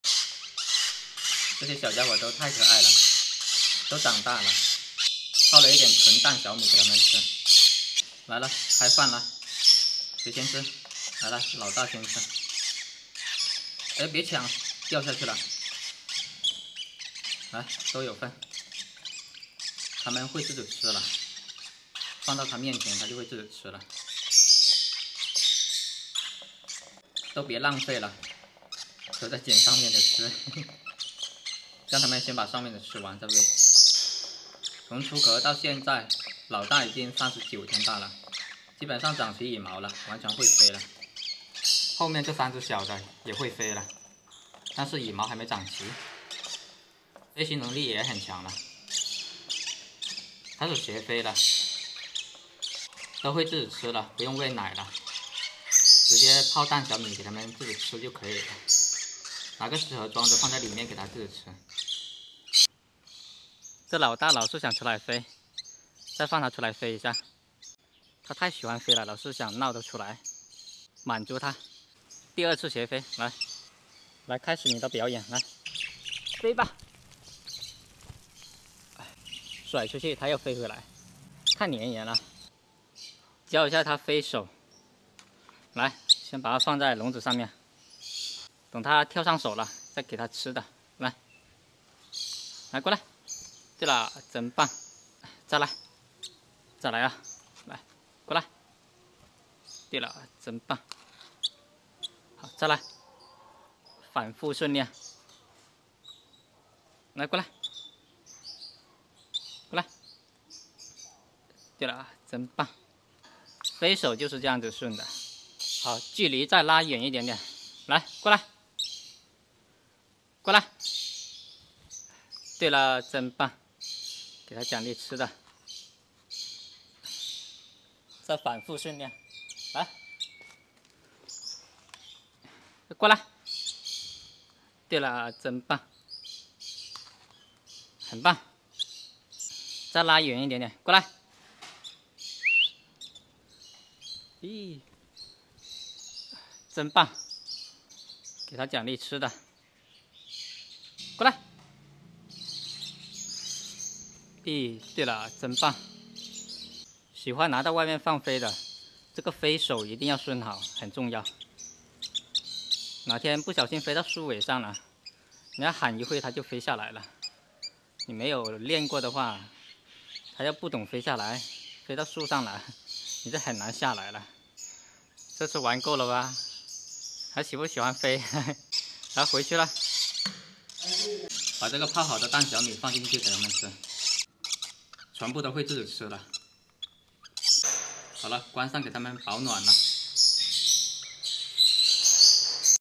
五只小家伙都会飞了！很可爱，很调皮！【叁伍萌宠】
"我们特别关心动物的幸福和安全，因此我们确保视频中没有任何形式的动物虐待。" 五只小家伙都会飞了！很可爱，很调皮！
喜欢大自然，专注鸟类，坚持原创视频
万物皆有灵，分享训鸟经验，养鸟知识